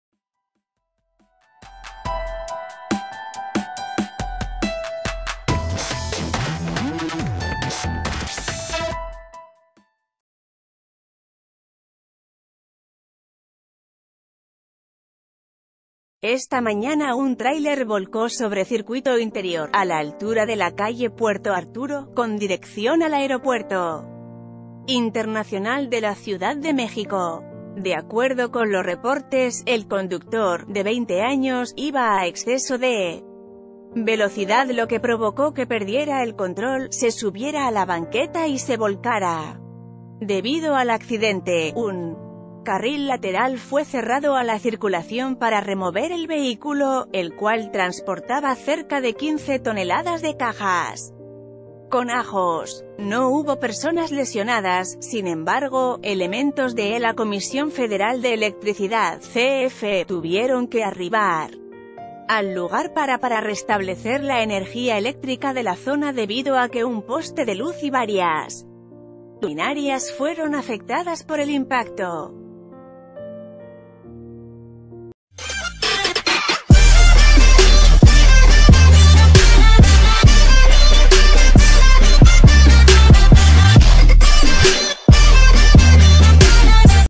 Por exceso de velocidad, tráiler vuelca en Circuito Interior
Esta mañana un tráiler volcó sobre Circuito Interior, a la altura de la calle Puerto Arturo, con dirección al Aeropuerto Internacional de la Ciudad de México. De acuerdo con los reportes, el conductor, de 20 años, iba a exceso de velocidad lo que provocó que perdiera el control, se subiera a la banq...